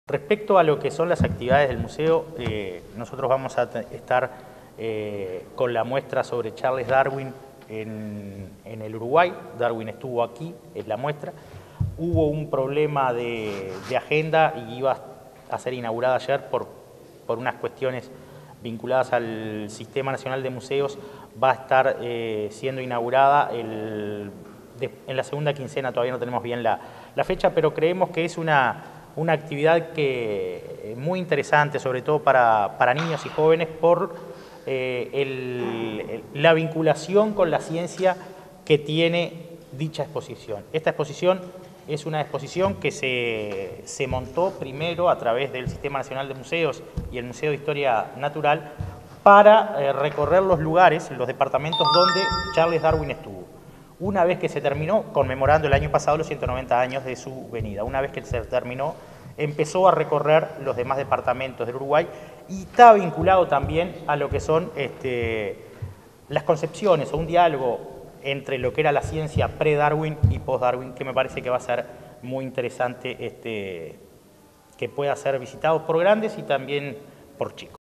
Guillermo Montaño
Lanzamiento de "Un Abril para Florida" 2024
Se realizó este martes 2 en el MAVEA – Centro Cultural Florida, una conferencia de prensa con motivo de brindar detalles de las actividades del programa “Un Abril para Florida 2024”, en el marco del 215º Aniversario de la Fundación de la Villa de San Fernando de la Florida.
Participaron de la conferencia el director general de Cultura, German Lapasta, la directora de Turismo, Ps. Carmen Pasarella, el director de Deportes, Mathias Domínguez, el director del Museo Histórico Departamental, Prof. Guillermo Montaño, la secretaria de Juventud, María José Astengo y la coordinadora del Núcleo Sinfónico de Florida Milka Gurgitano.